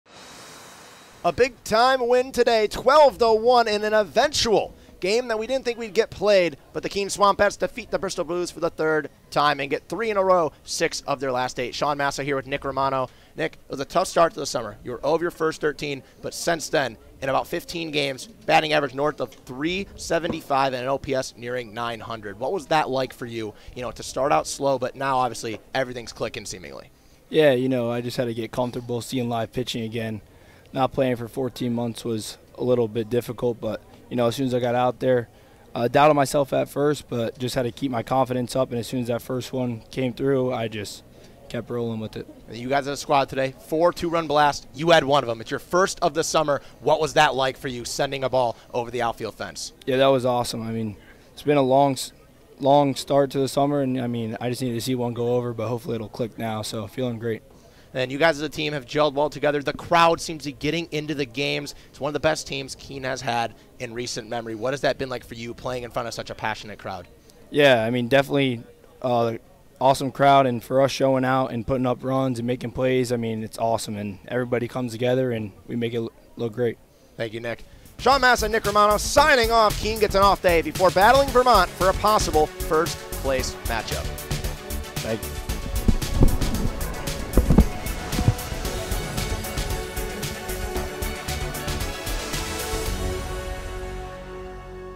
Nick Romano Postgame Interview 7/6/2024
The Florida Atlantic Owl talks about his torrid stretch at the plate and his first home run as the Bats got early separation and stomped Bristol 12-1.